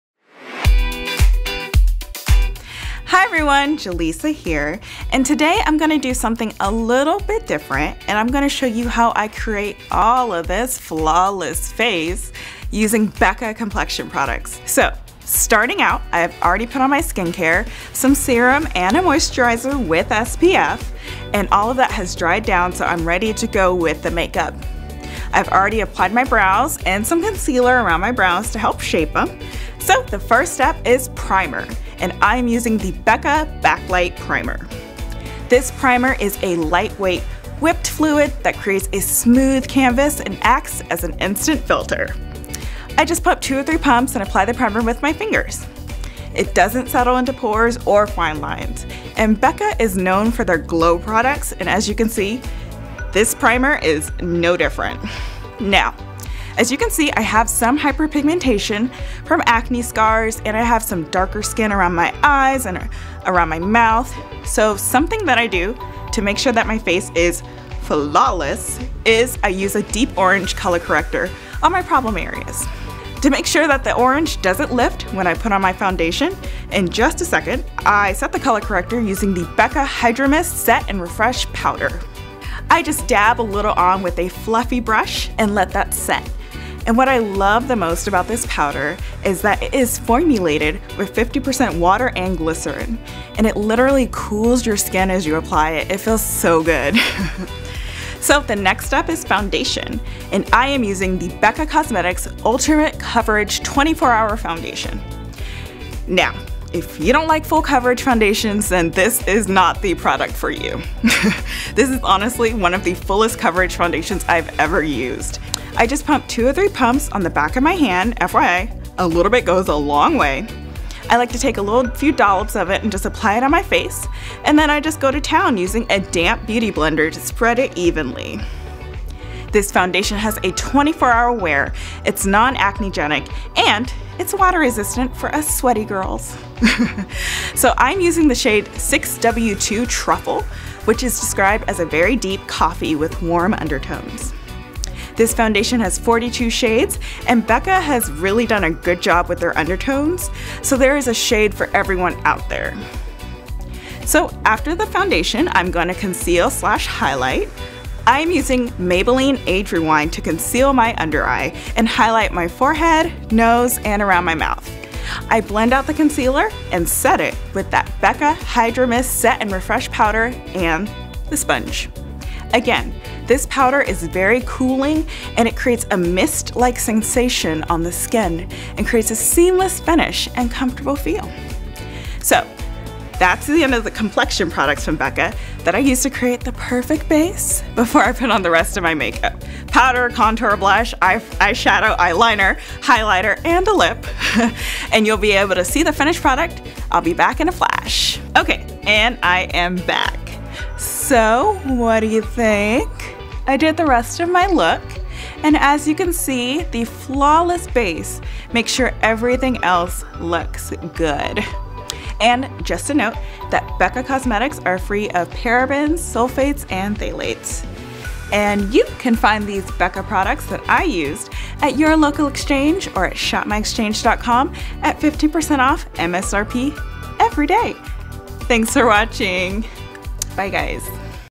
Becca Foundation How-To
Shoutout to all the Becca Beauties out there!
Here's how our beauty expert, Jalessa applies her foundations for a flawless face!